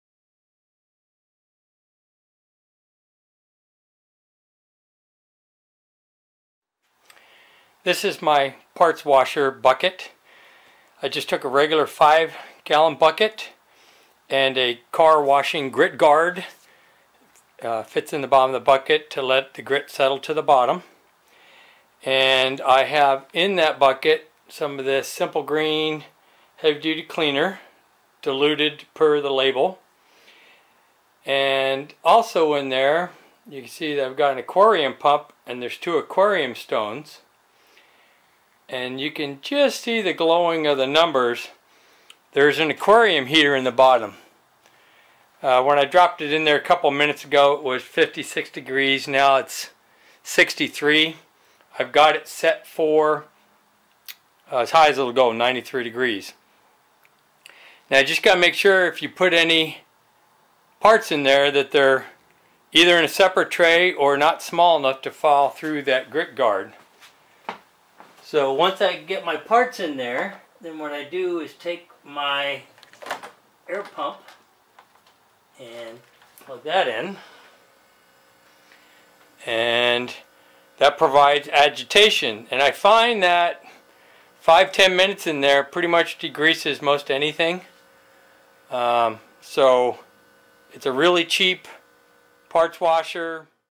Cheap 5 gallon bucket parts washer
A short video showing how to make a parts washer out of a 5 gallon bucket.